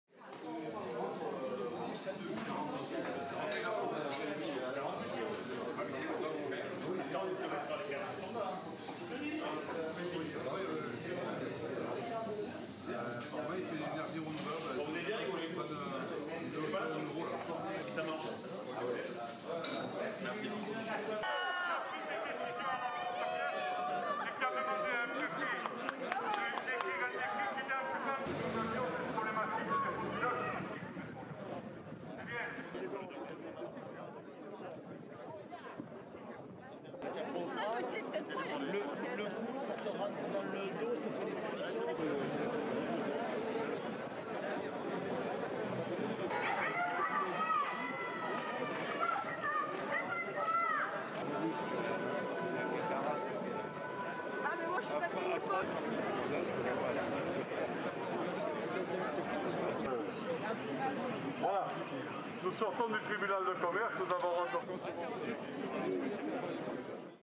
Bar workers in Marseille protest as week long closure enforced
Restaurants and bars in Marseille are set to close for a week as French ministers launch scattered new coronavirus restrictions in a bid to stem a rising tide of infections. Hospitality businesses in the Mediterranean-coastal city will close for seven days from tonight. It comes after health authorities reported 14,000 new infections across France on Saturday - amid a mass testing effort. Meanwhile hospitals in the Paris and Marseille regions will put scheduled operations on hold to free up space for a growing number of Covid-19 patients.